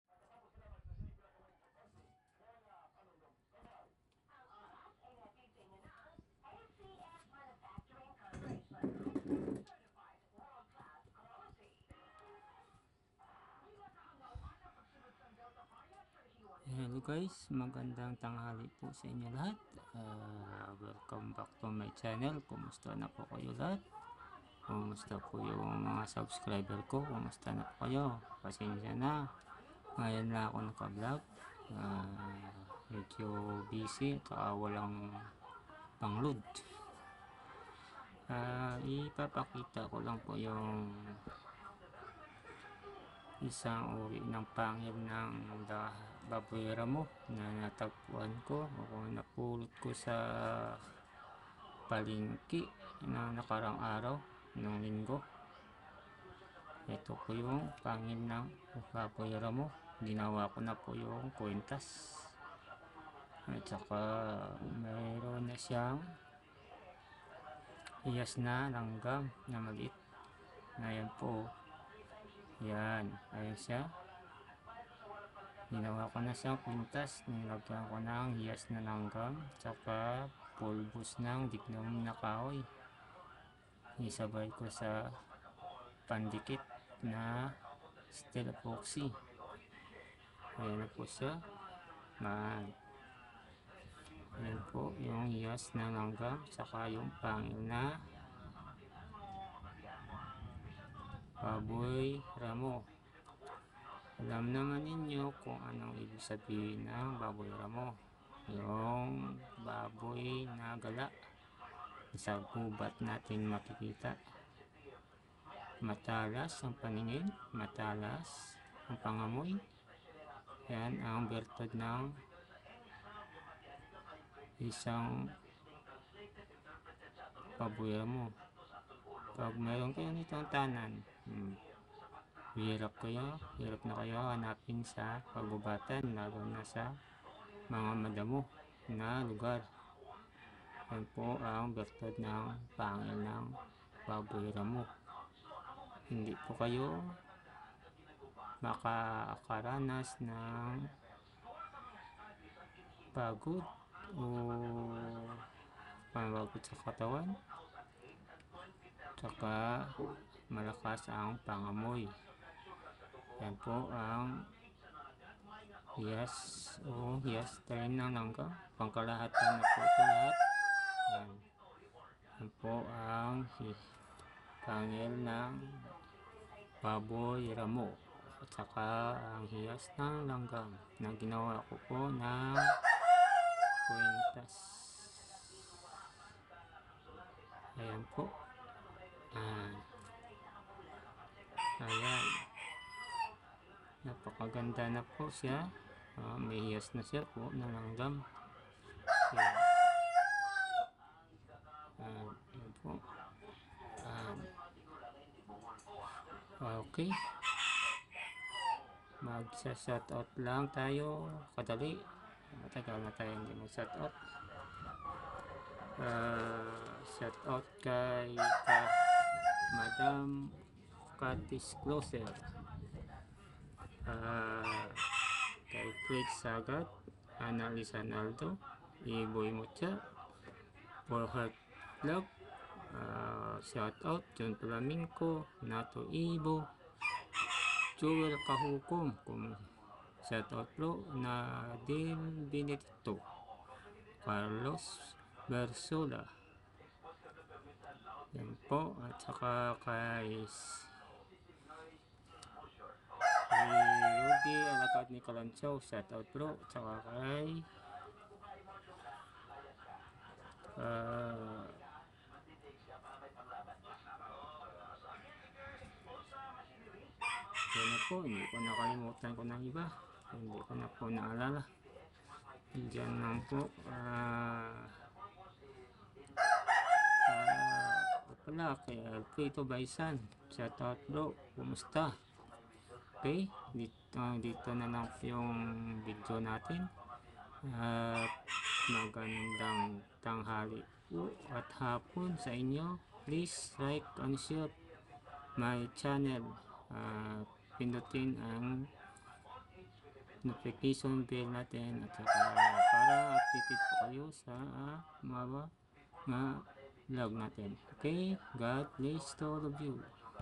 September 14, 2021
pangil ng baboy ramo gnawng kwentas Na my ksama hiyas ng langgam at polbos Na dignum kahoy